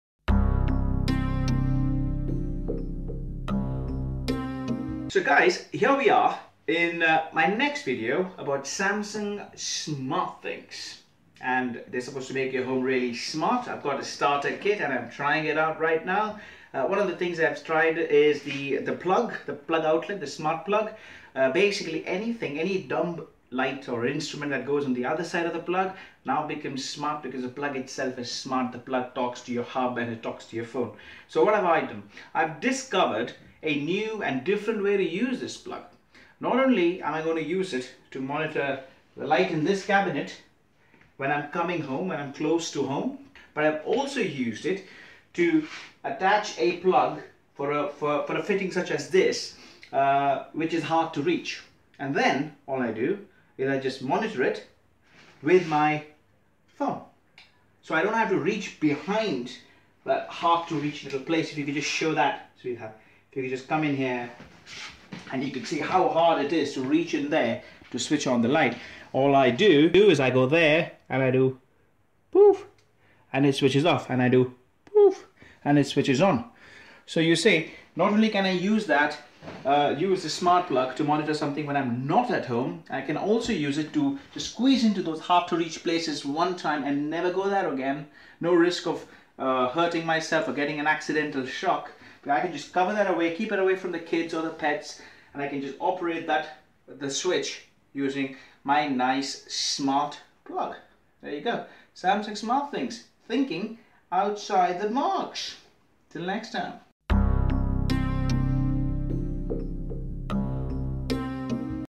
Smart way to use the Samsung Smart Things Power Outlet
Exploring a whole new reason to start using the Samsung Smart Things Power Outlet. Get smart and make a smart move! Watch the video and find out how.